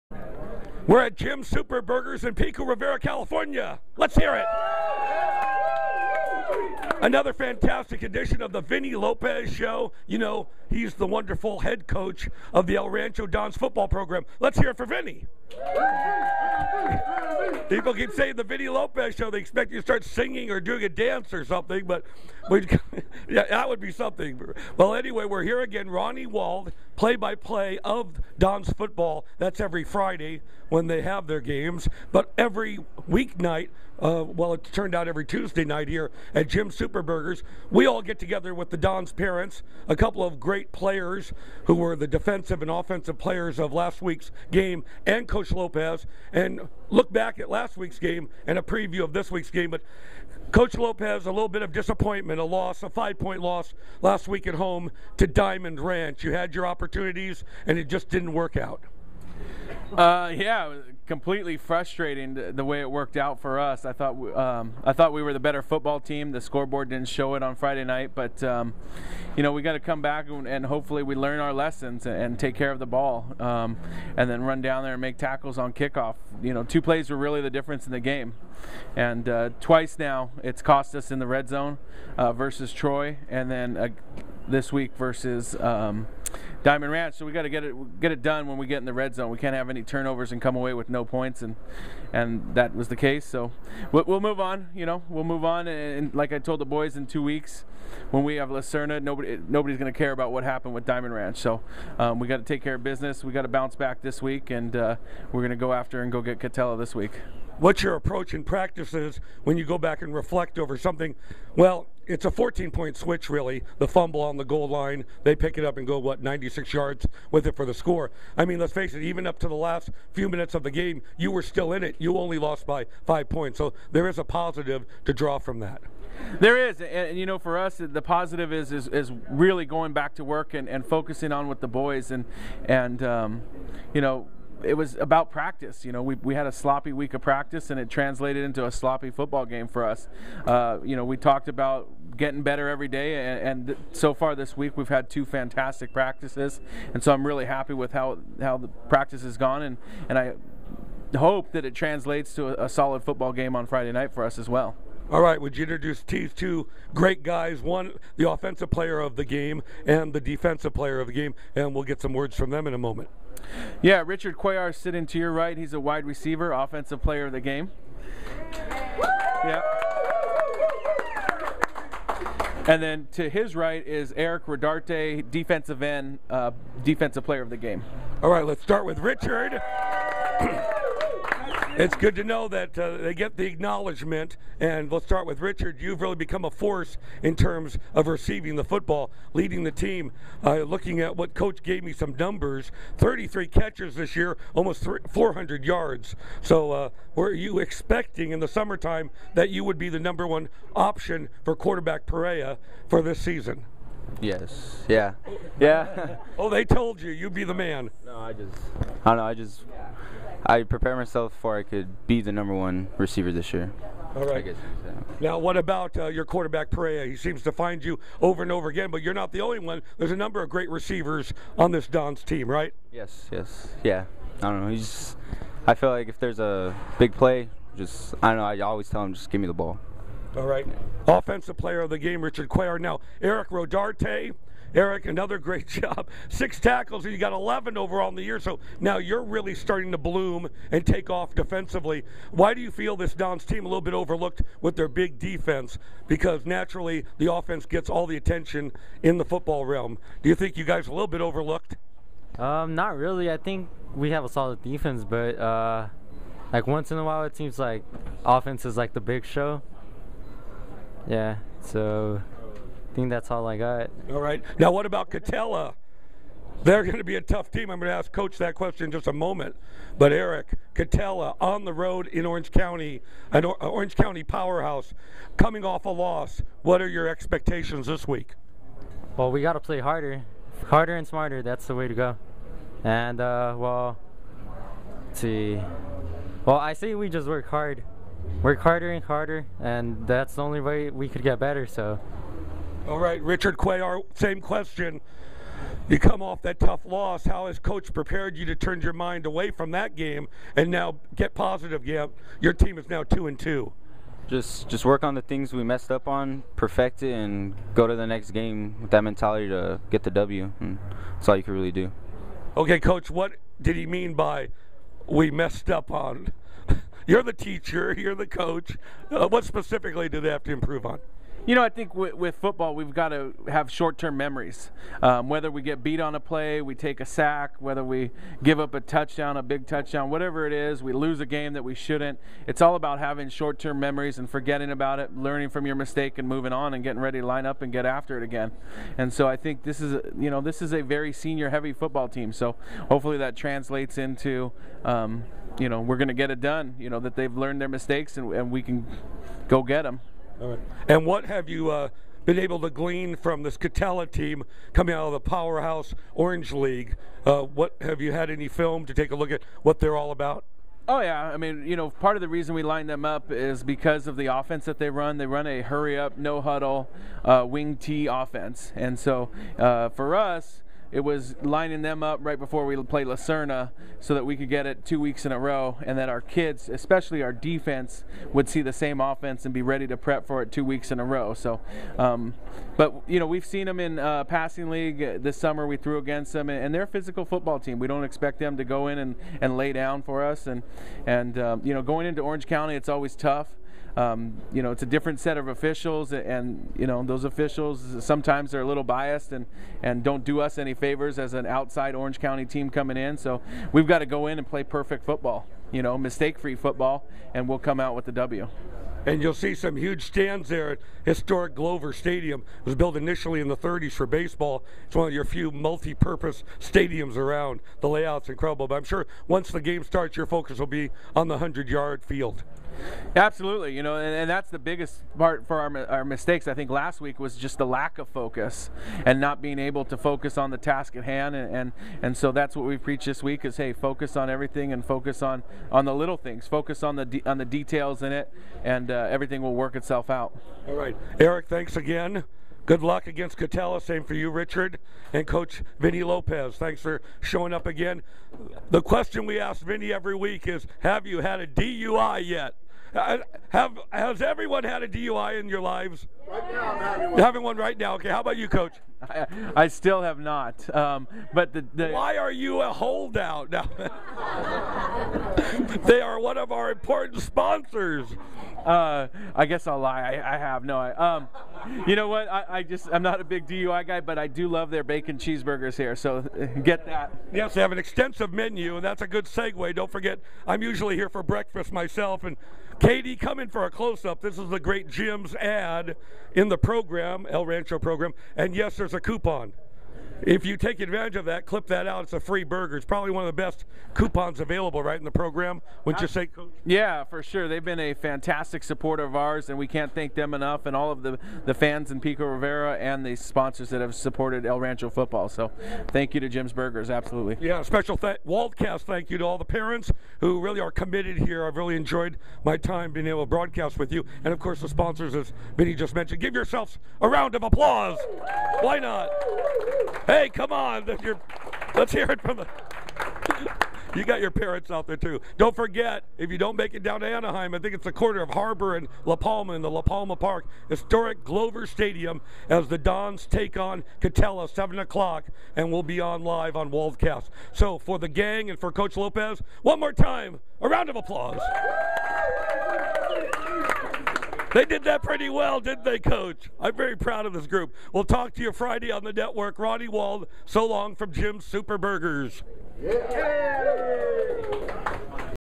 Vinnie Lopez Show 4 El Rancho Football 2014 by Waldcast
Hosted by Ronnie Wald--Broadcaster. At Jim's Super Burgers in Pico Rivera, Calif.